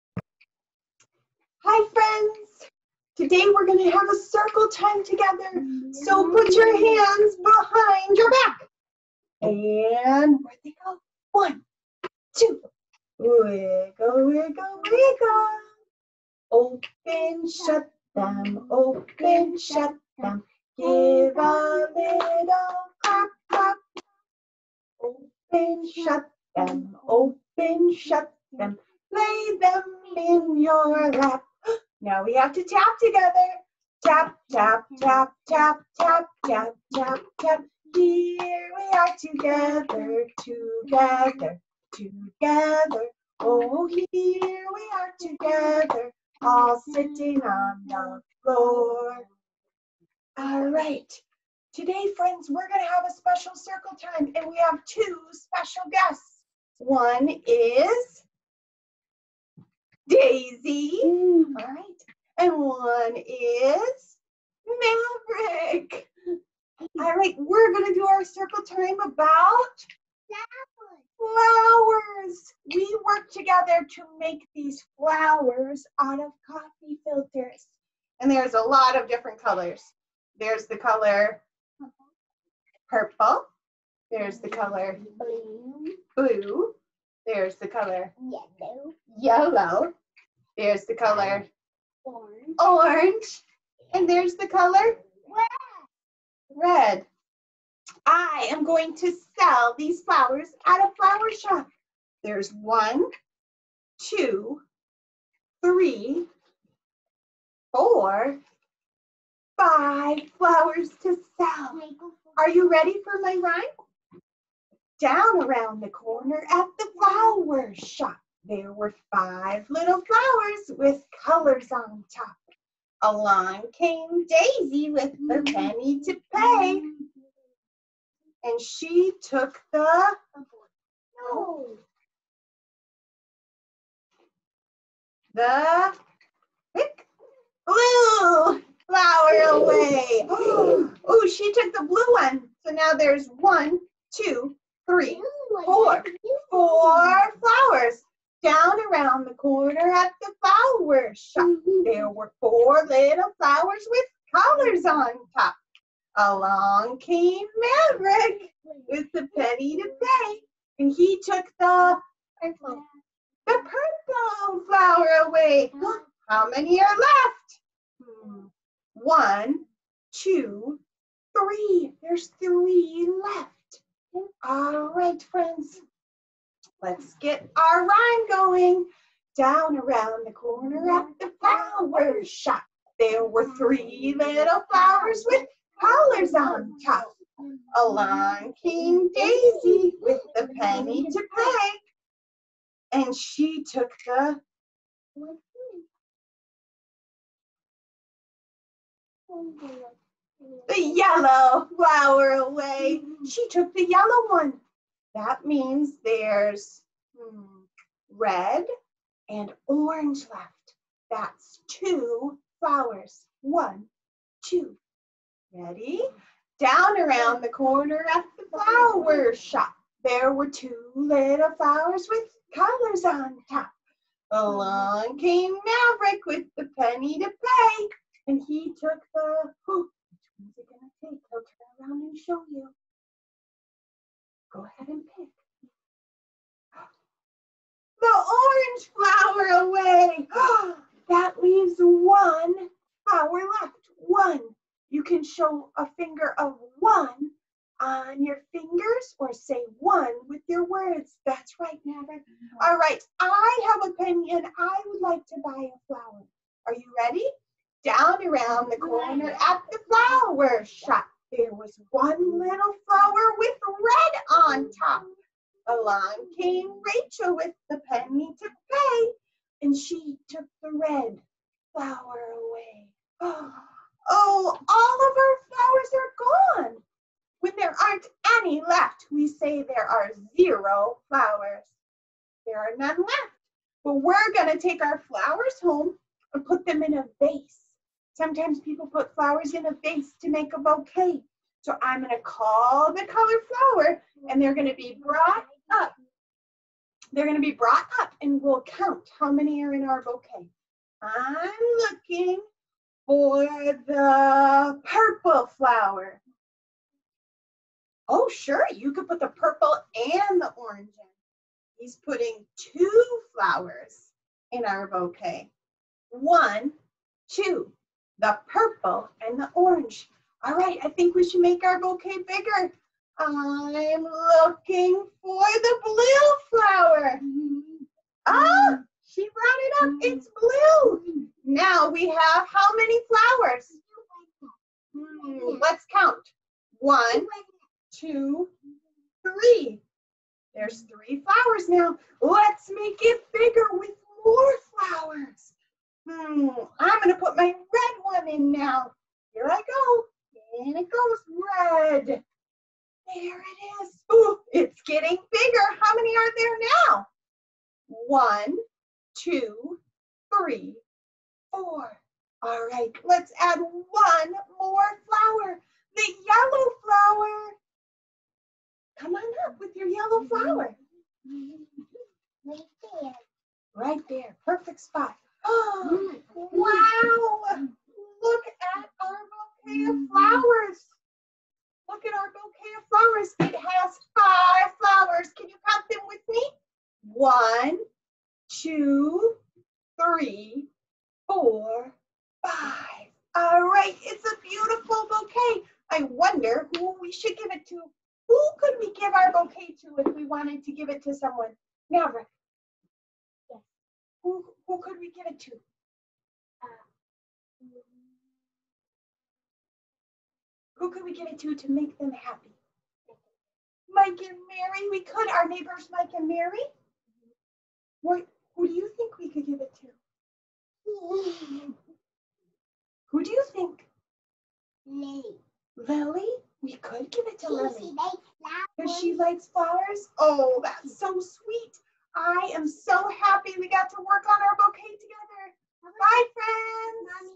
Five Little Flowers
Five colorful flowers are for sale at the flower shop. I wonder who will buy them?
Featuring my children Maverick and Daisy...
Maverick called the purple flower before we even started so to avoid a fight we planned out which flowers each of them would buy. Needless to say that was a wasted plan.

Enjoy! Just keeping it real.